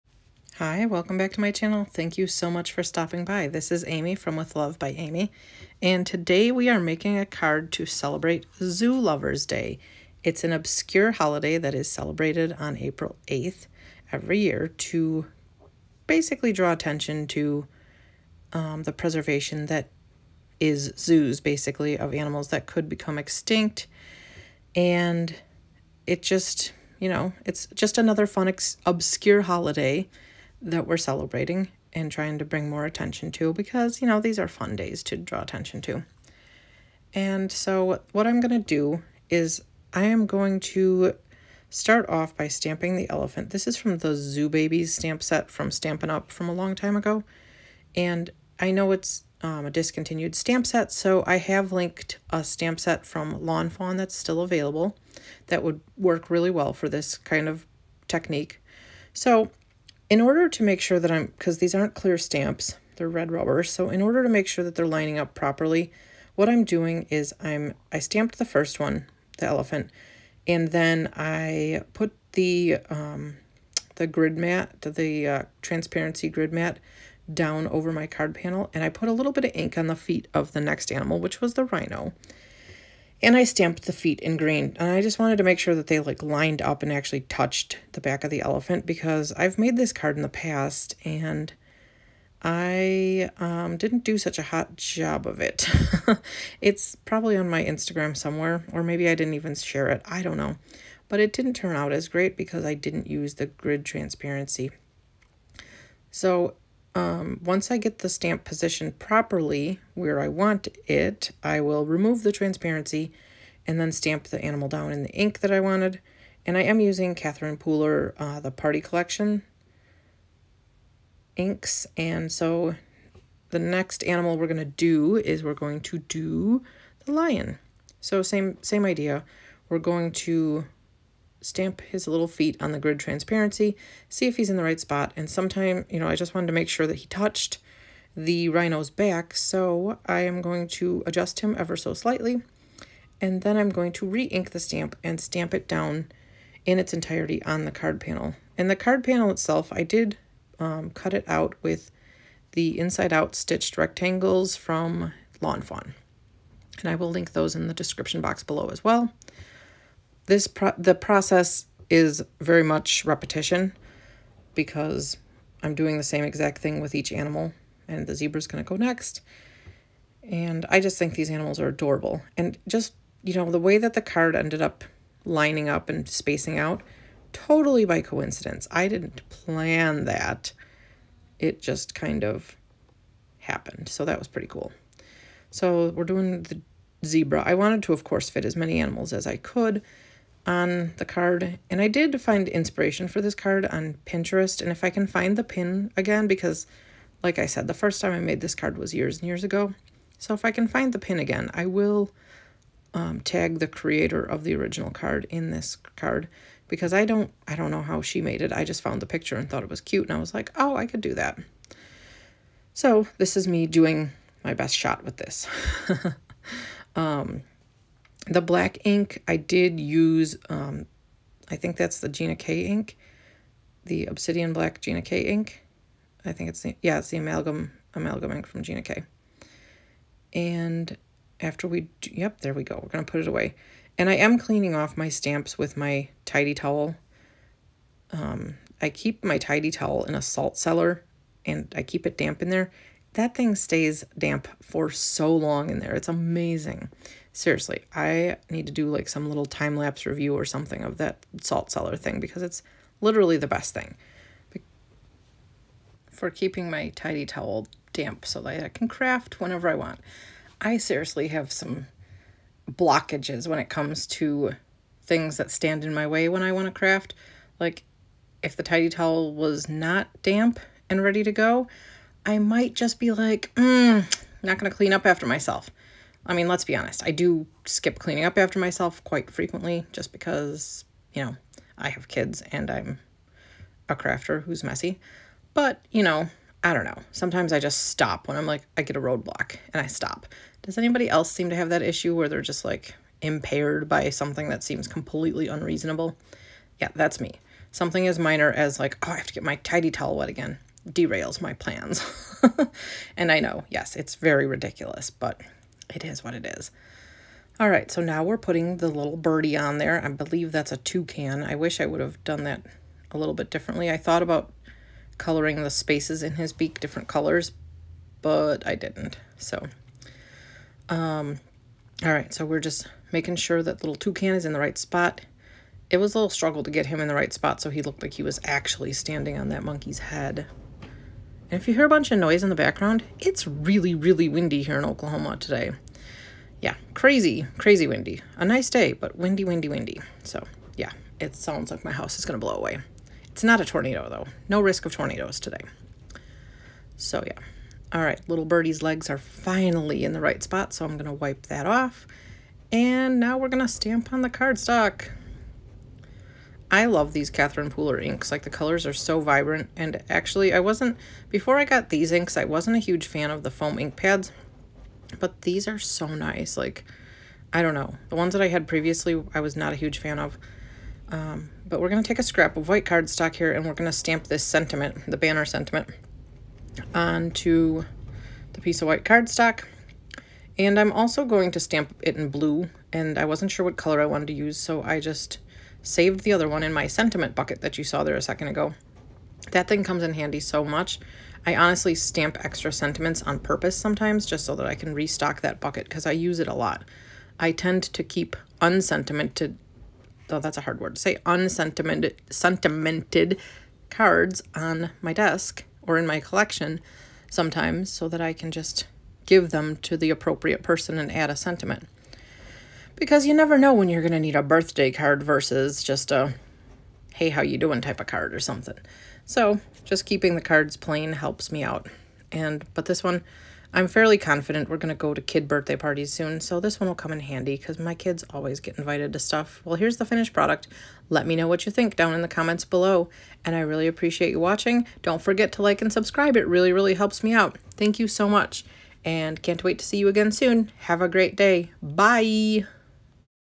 Cute zoo animals card
Zoo lovers day is April 8th. Did you know that there are over 2,400 zoos in the United States?
Did you know that the earliest recorded zoo was 1500 B.C.? Zoos help bring people closer to wildlife as well as help preserve endangered species. We have a membership to our local zoo, and my kids love going as often as we can!